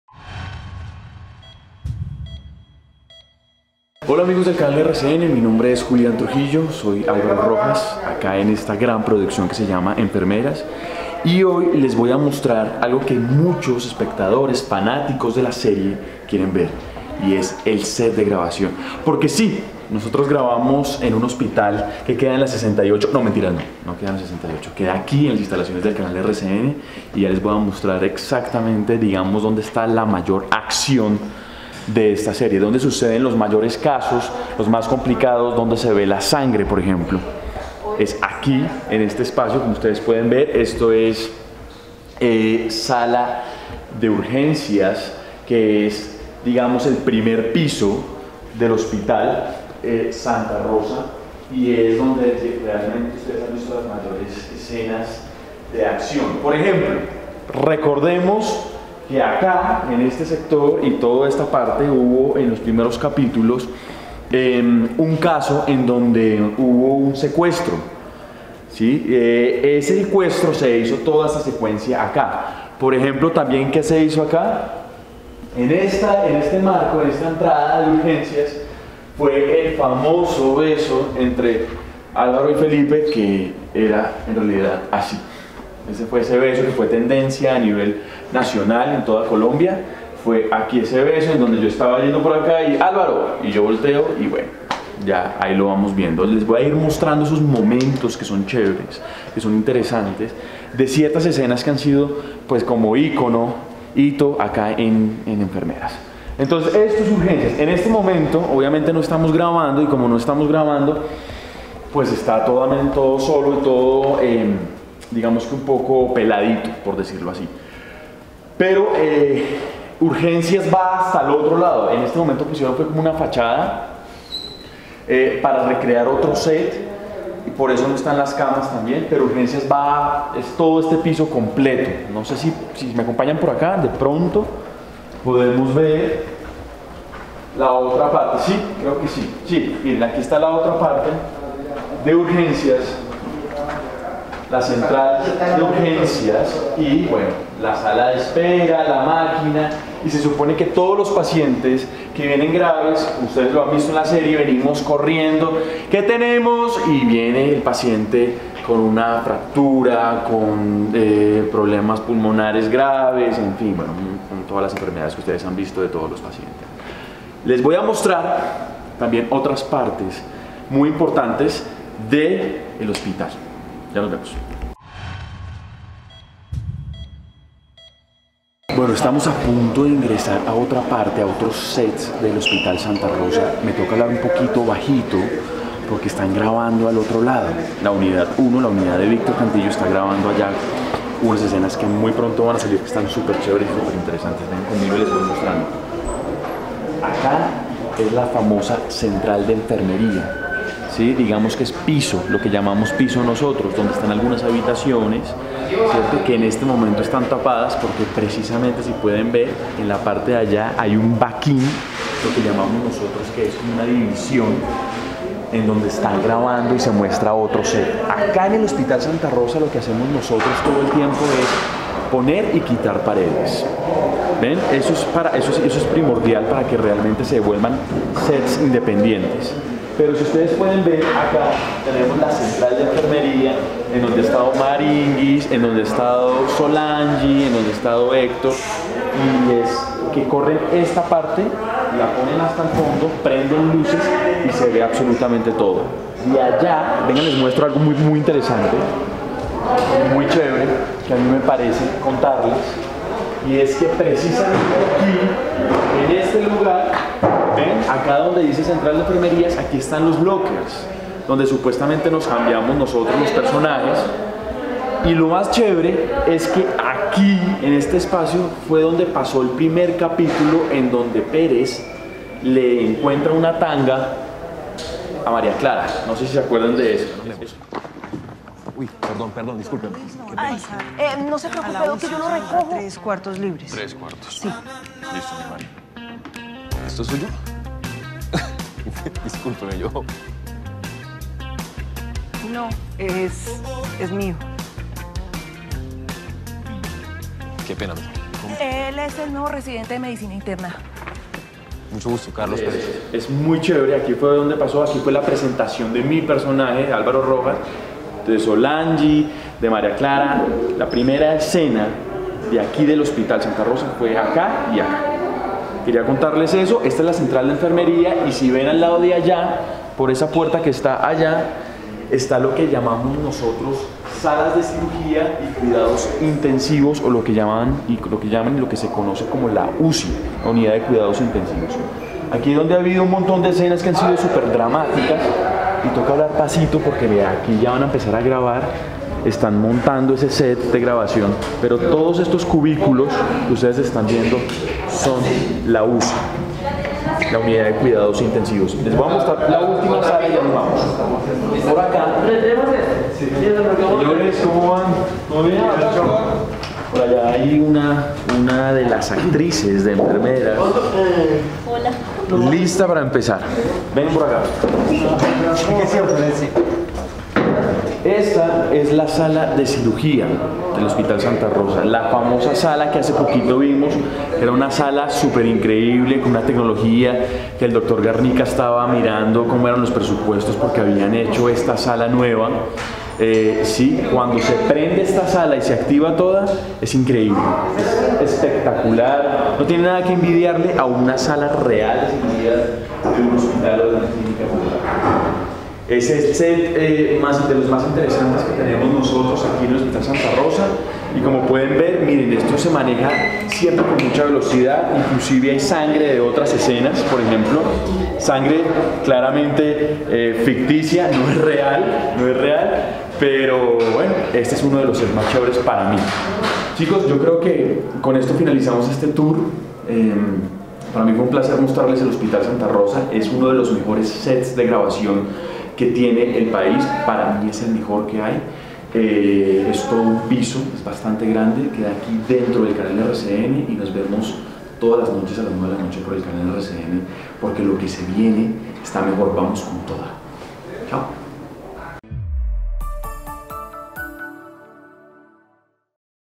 Recorre las INSTALACIONES DE HOSPITAL SANTA ROSA CON Julián Trujillo | Enfermeras
Para los que se preguntan: ¿El hospital Santa Rosa es de verdad?, ¿Dónde queda el hospital?, ¿Cómo es el hospital de Enfermeras por dentro? Y más, Julián Trujillo los sacará ya mismo de las dudas.